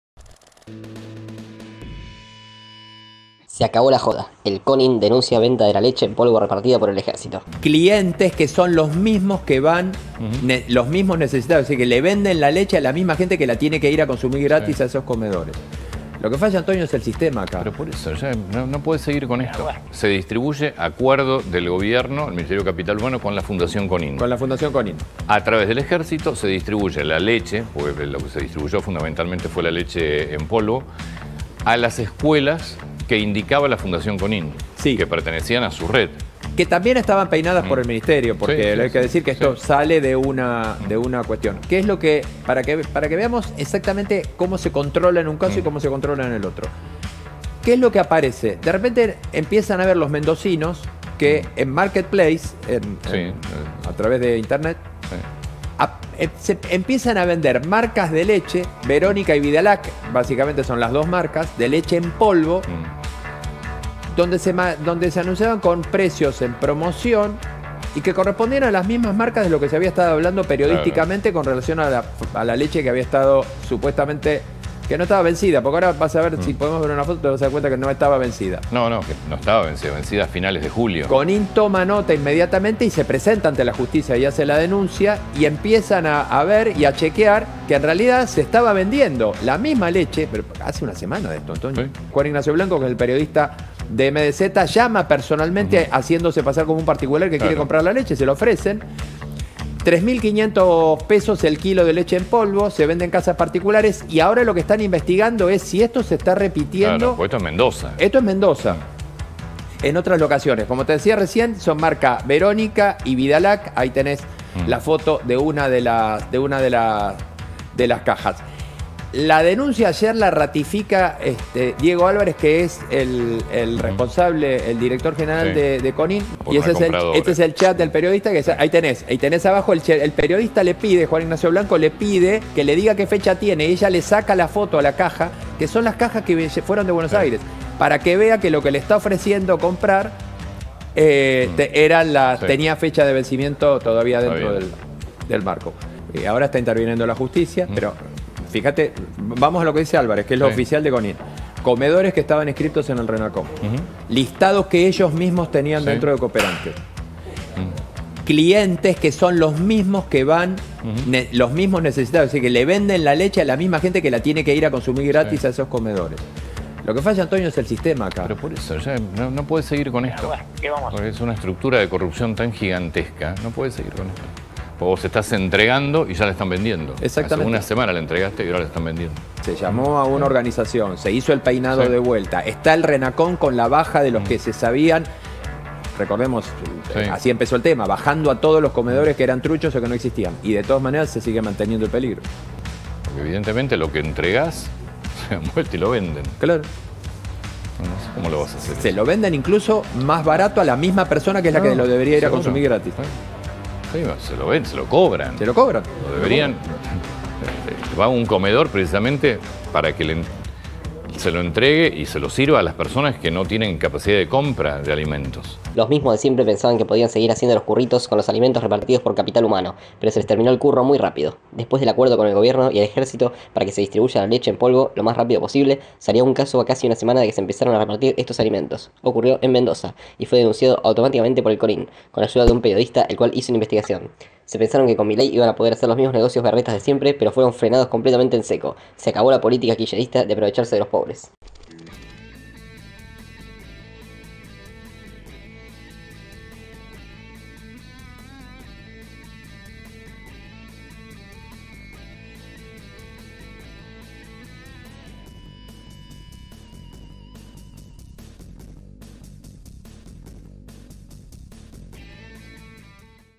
SE ACABÓ LA JODA: El CONIN denuncia venta de la leche en polvo repartida por el ejercito
Los mismos de siempre pensaban que podían seguir haciendo los curritos con los alimentos repartidos por Capital Humano, pero se les terminó el curro muy rápido. Después del acuerdo con el Gobierno y el ejercito para que se distribuya la leche en polvo lo más rápido posible, salió un caso a casi una semana de que se empezaron a repartir estos alimentos, ocurrió en Mendoza y fue denunciado automáticamente por el CONIN, con ayuda de un periodista el cual hizo la investigación. Se pensaron que con Milei iban a poder hacer los mismos negocios berretas de siempre pero fueron frenados completamente en seco, se acabó la política kirchnerista de aprovecharse de los pobres